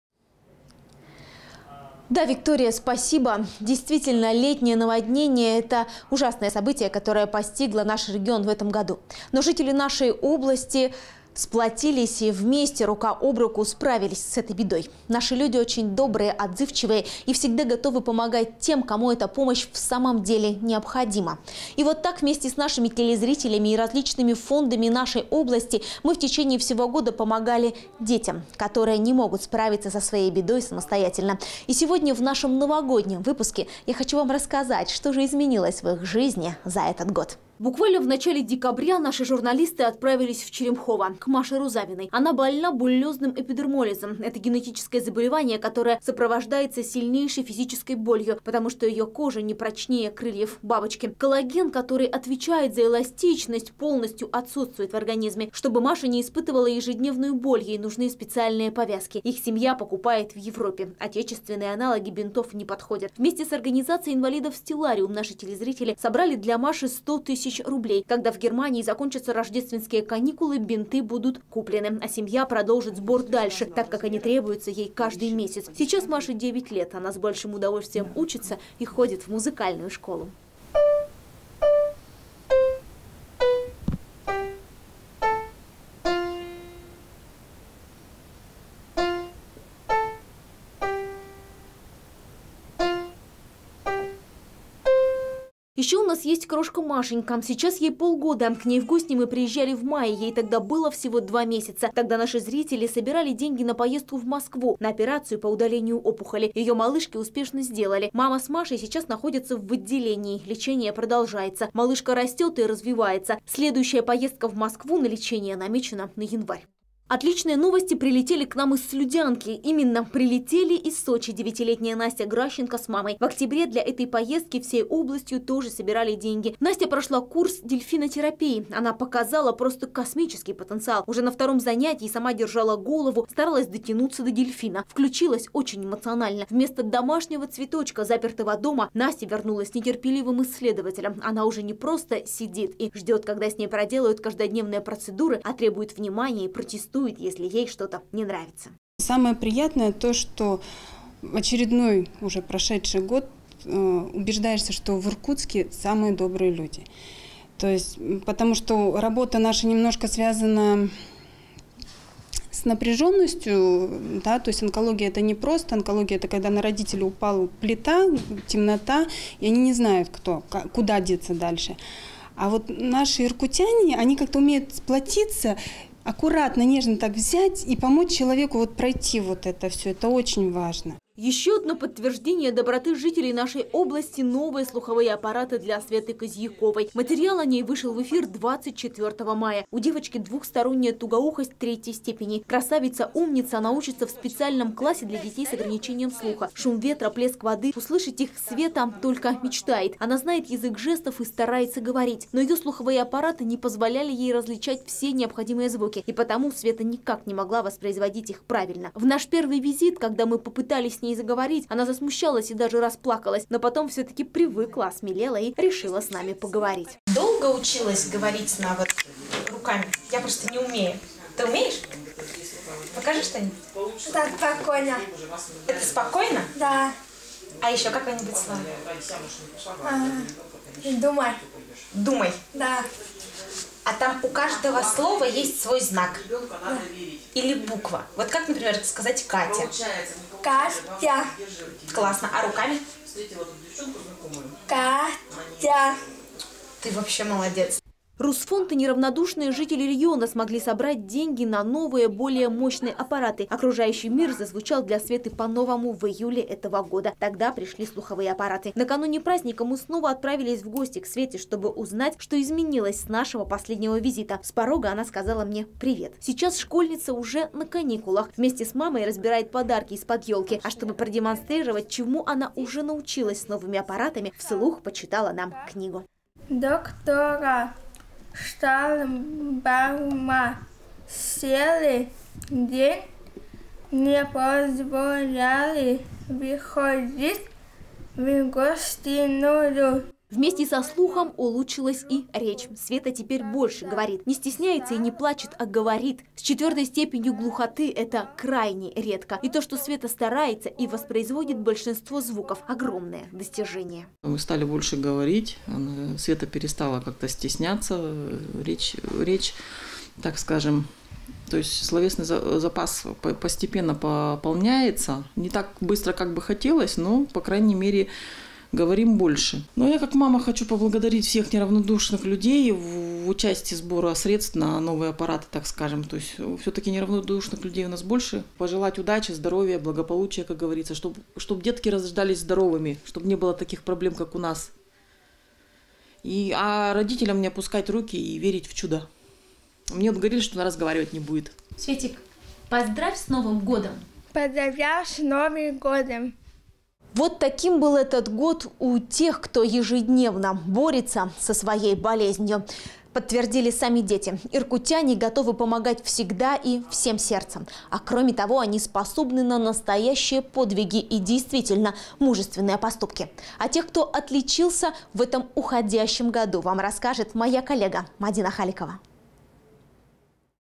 Помощь людям
Летнее наводнение ужасное событие, которое постигло наш регион. Но жители нашей области выстояли и справились с этой бедой все вместе, помогая друг другу. Наши люди очень добрые и отзывчивые и всегда готовы прийти на помощь в трудную минуту. В течение всего года мы с разными благотворительными фондами помогали детям, которым никак не справиться со своей бедой самостоятельно. Сегодня мы расскажем о том, что изменилось в их жизни за этот год.